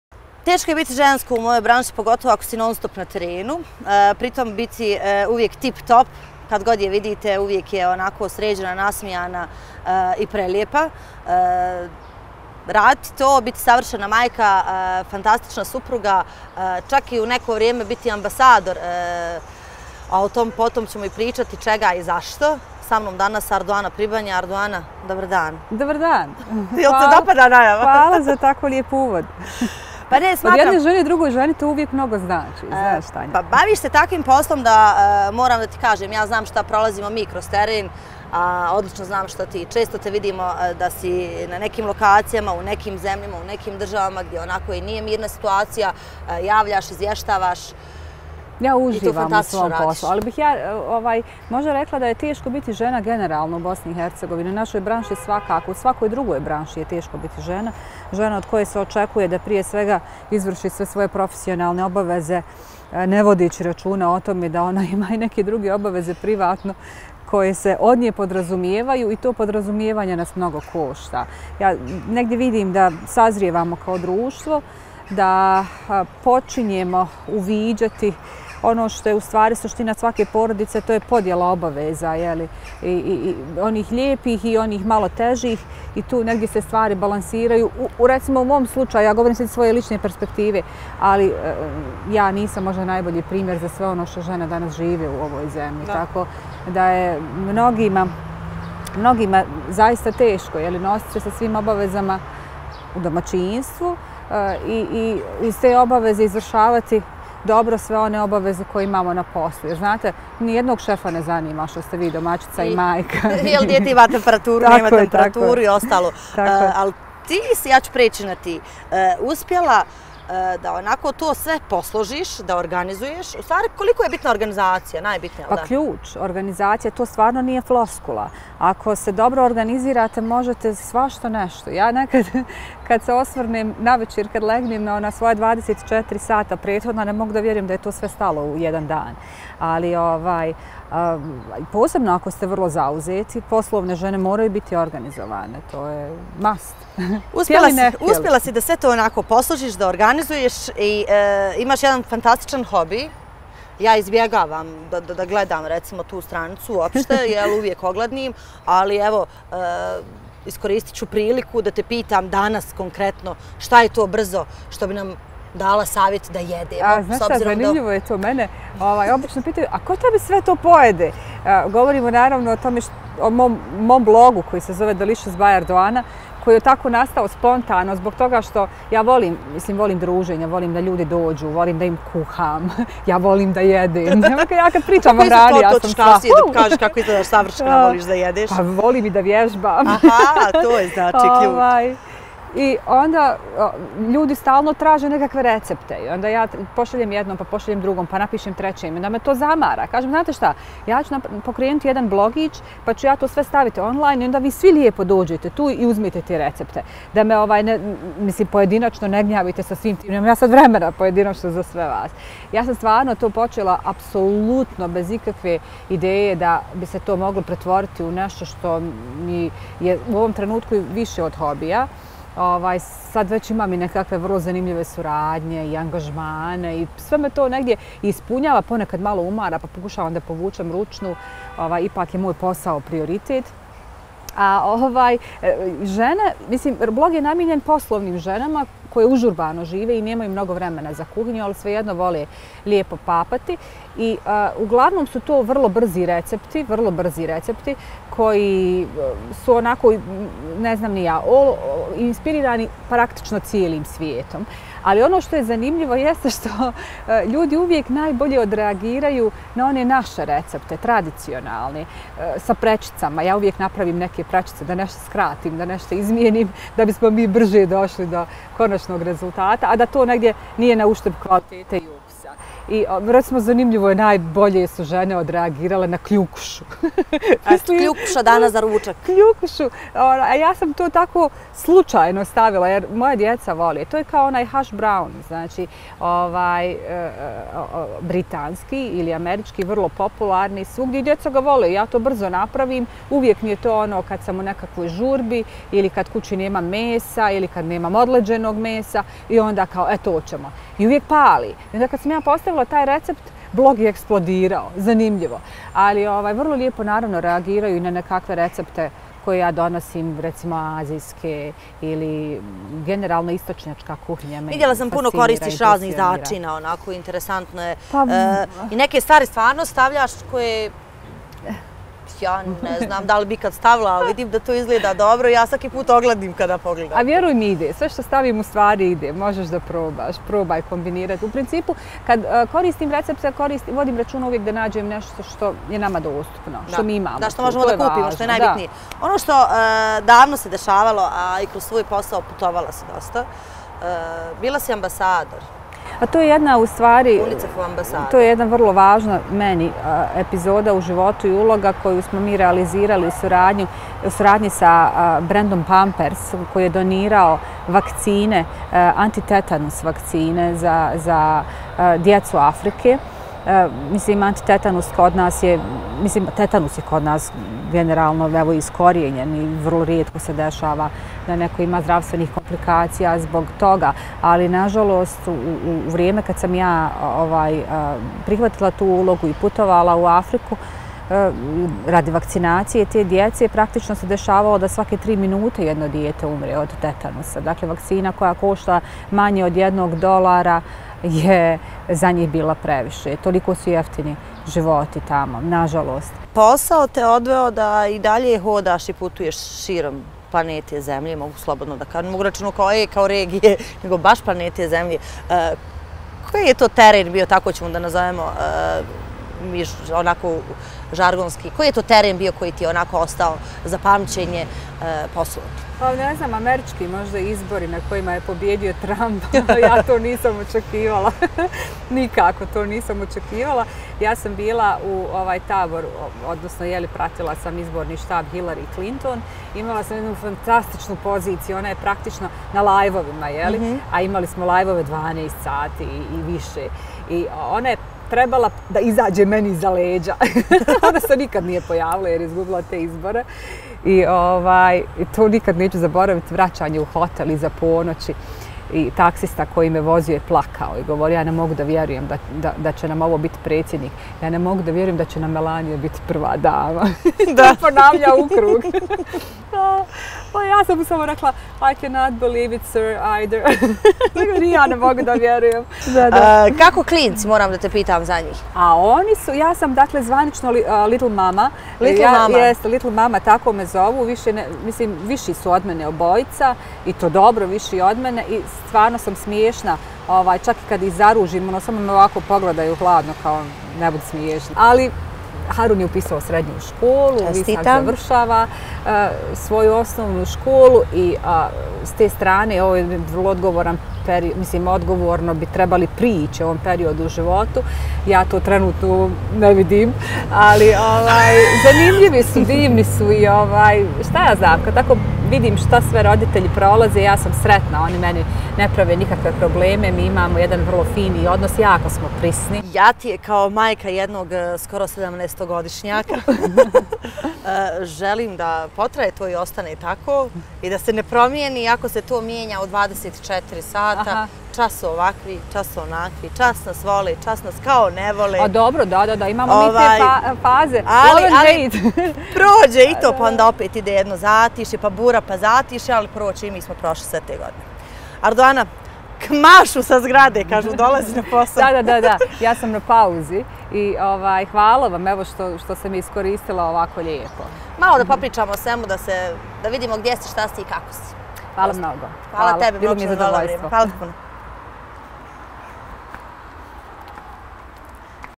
Arduna Pribinja i kulinarstvo
Arduana Pribinja novinarstvom se bavi već 20 godina. Pored ove ljubavi prije dvije godine rodila se još jedna, a to je kulinarstvo. Na njenom blogu ljudi različitih prehrambenih navika pronalaze nešto za sebe. Ljubav prema hrani Arduana je naslijedila od svoje majke koja je, kako kaže, krasno kuhala i bila pravi gurman.